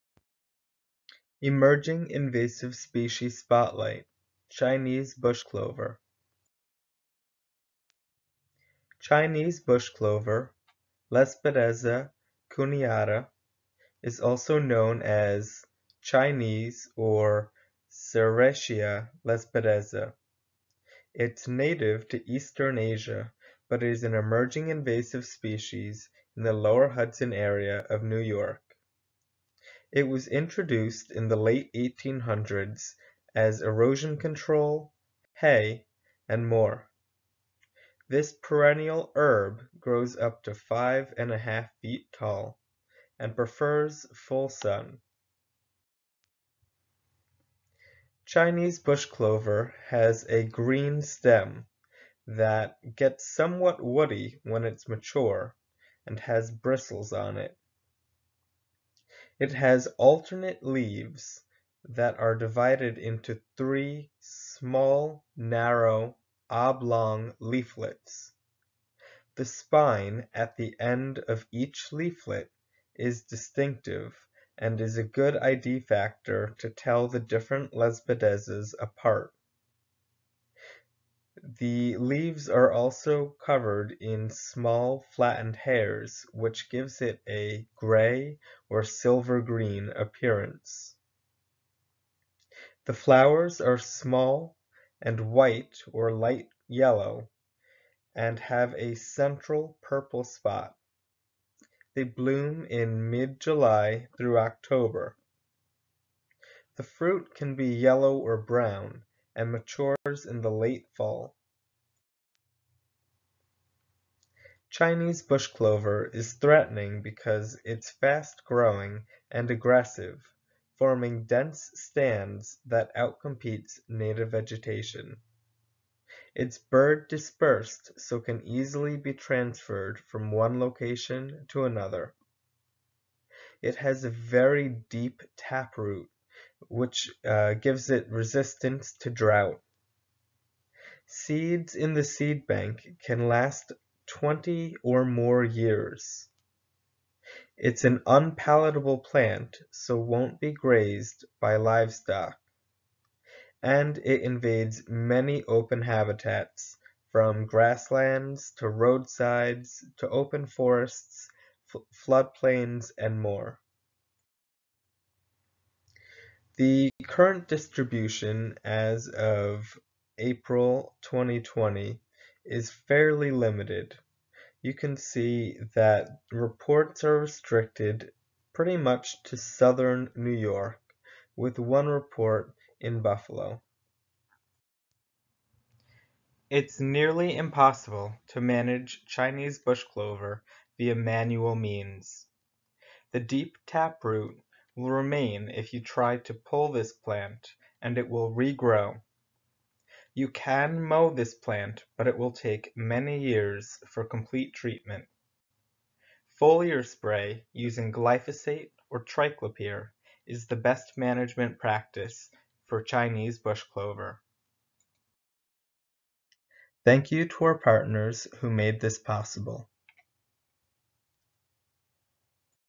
Chinese Bushclover Species Spotlight
Chinese bushclover is a perennial herb that is an emerging  invasive species in the Lower Hudson PRISM region. This video describes its ecology and ecological impacts, key ID features, and management.